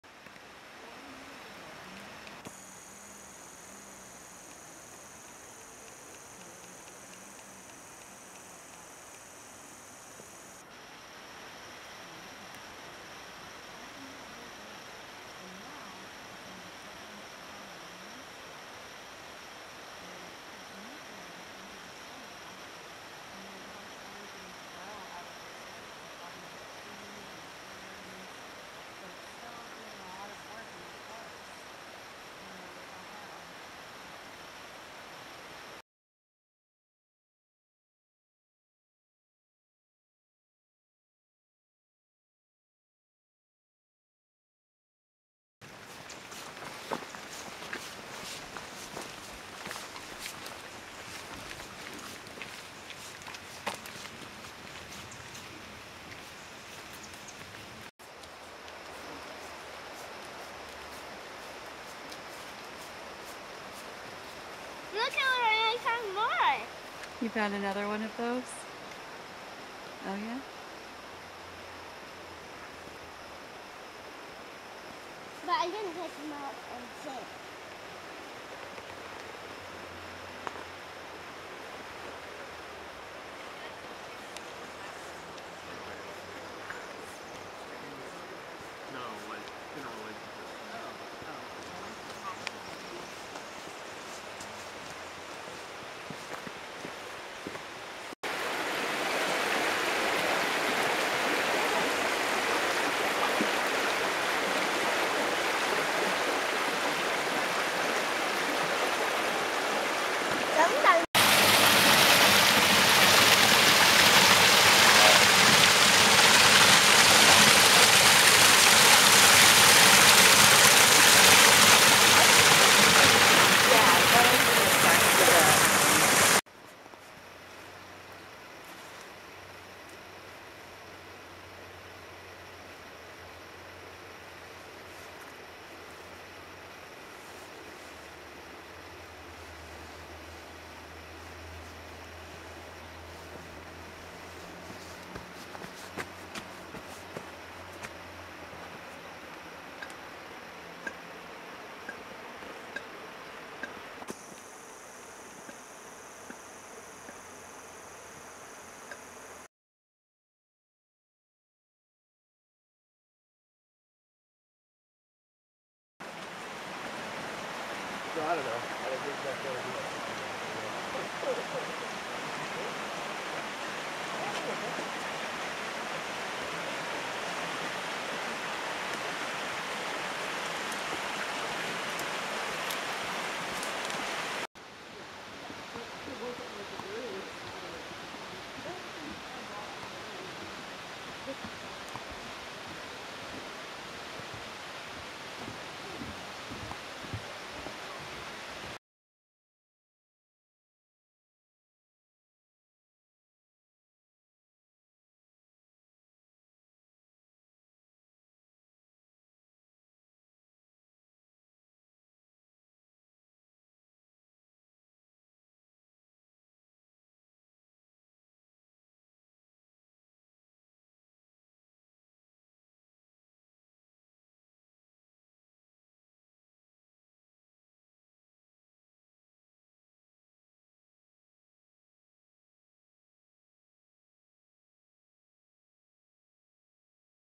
Muir Woods!
Muir Woods very early in the morning, 25 February 2017.